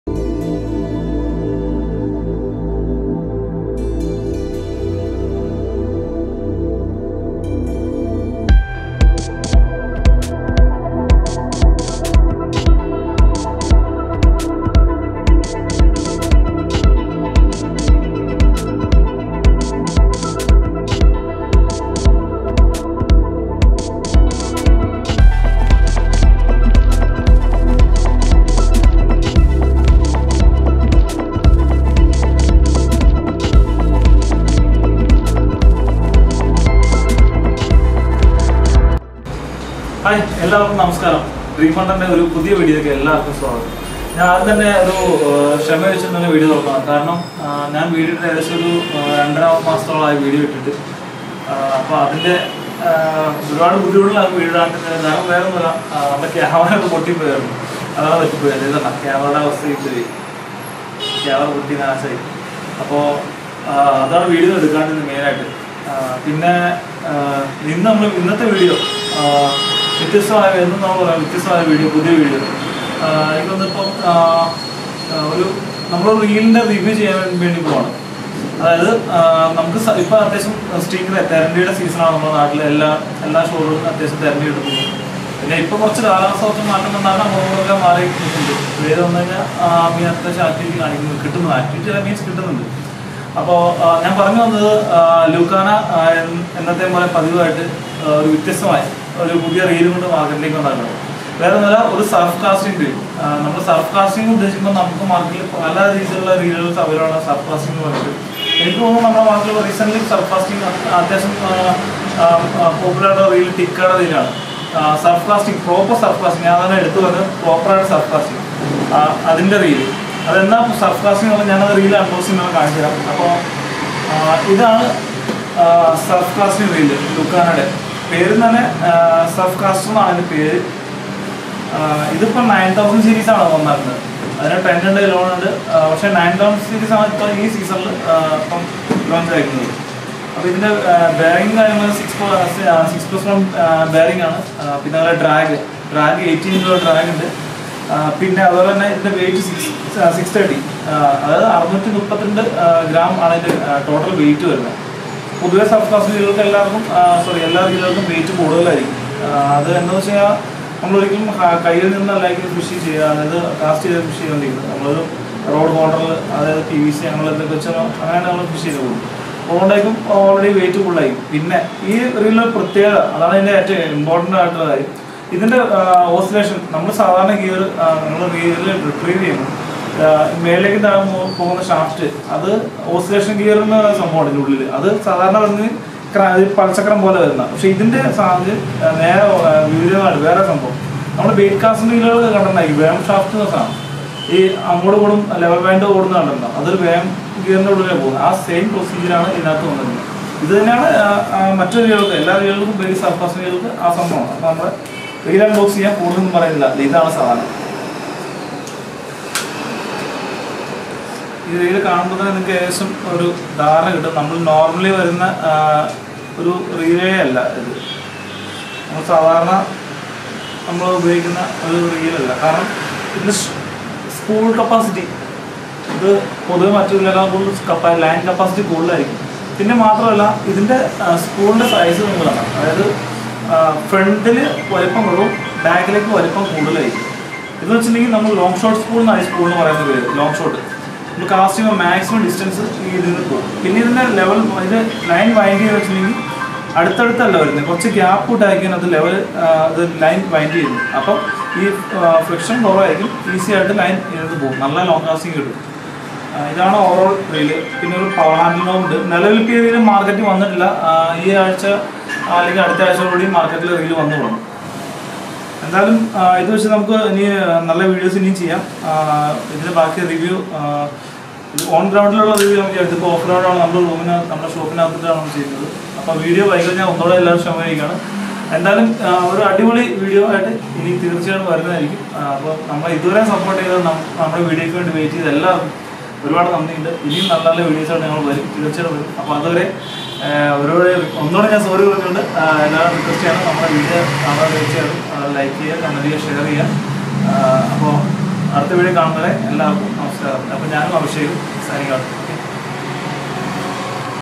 Long shot Surf Casting Reel From Lucana
Brand . Lucana
Reel Name  Surfcaster
Reel Size 9000 series
Drag weight .18 kg
Power handle Knobe